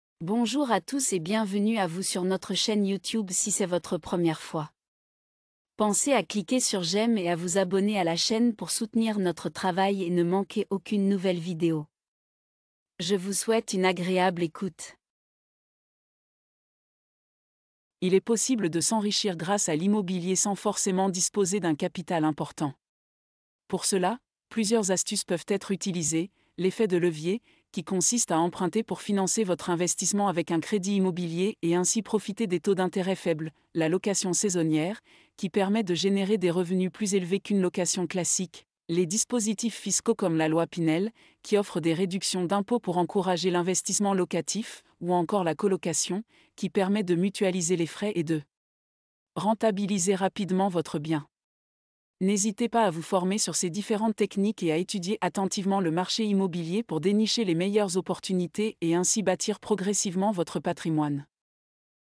DEVENEZ RICHE GRACE A CES ASTUCES POUR ACHETER DES BIENS SANS ARGENT
Il est possible de s'enrichir grâce à l'immobilier sans forcément disposer d'un capital important. Pour cela, plusieurs astuces peuvent être utilisées : l'effet de levier, qui consiste à emprunter pour financer votre investissement avec un crédit immobilier et ainsi profiter des taux d'intérêt faibles ; la location saisonnière, qui permet de générer des revenus plus élevés qu'une location classique ; les dispositifs fiscaux comme la loi Pinel, qui offrent des réductions d'impôts pour encourager l'investissement locatif ; ou encore la colocation, qui permet de mutualiser les frais et de rentabiliser rapidement votre bien. N'hésitez pas à vous former sur ces différentes techniques et à étudier attentivement le marché immobilier pour dénicher les meilleures opportunités et ainsi bâtir progressivement votre patrimoine.